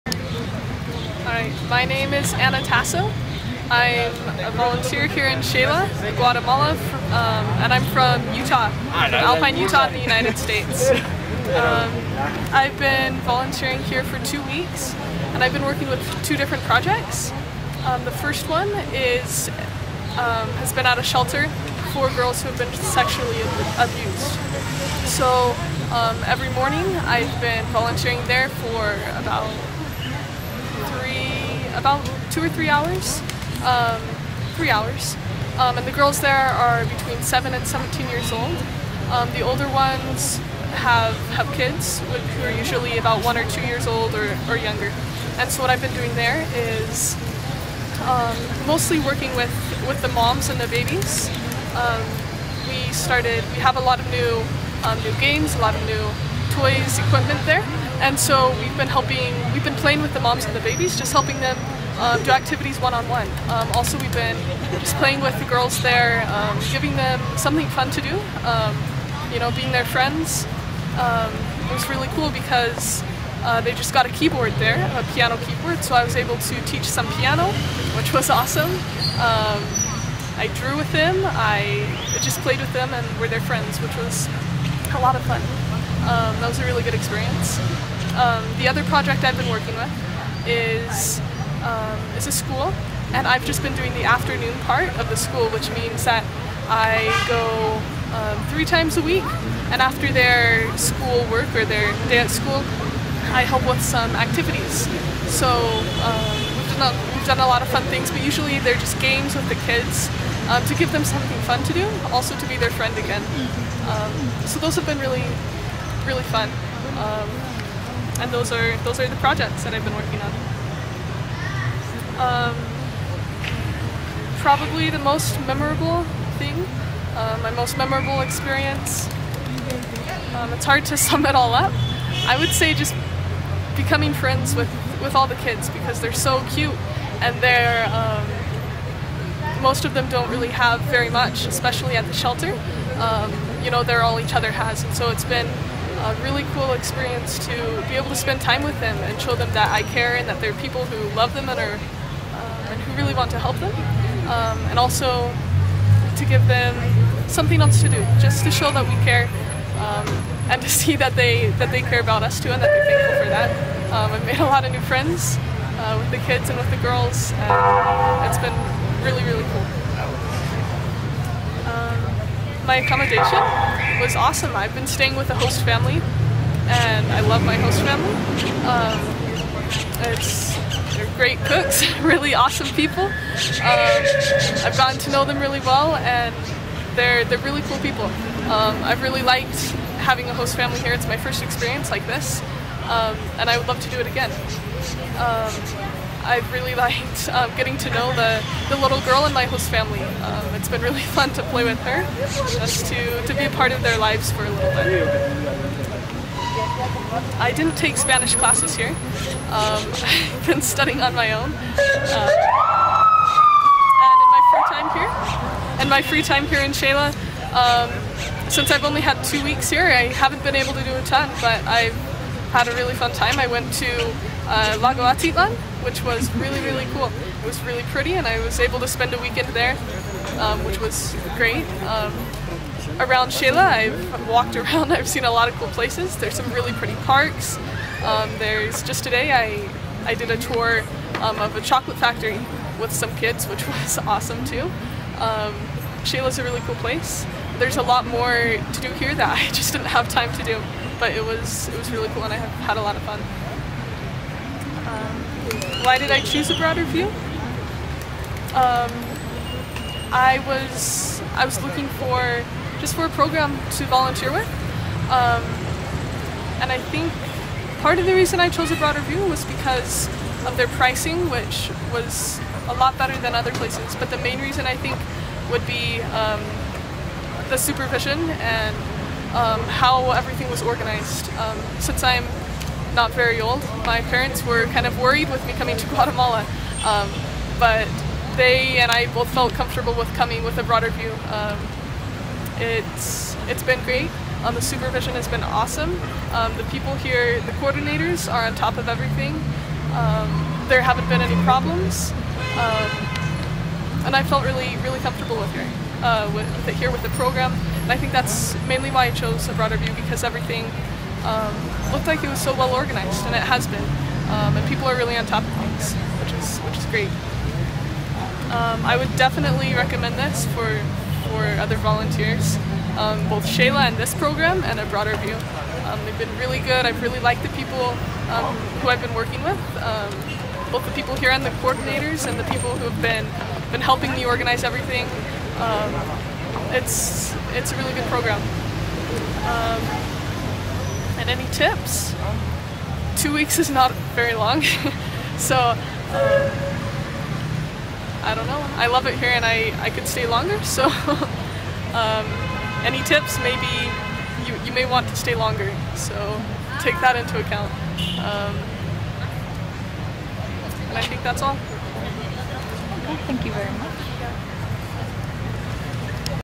HD Video Feedback Volunteer Name: Anna Tasso - Project Location: Xela, Guatemala
HD Video Feedback Volunteer Name: Anna Tasso - Project Location: Xela, Guatemala -  Dates: From 25/7 to 8/8/2014 - Volunteered at: Orphanage / shelter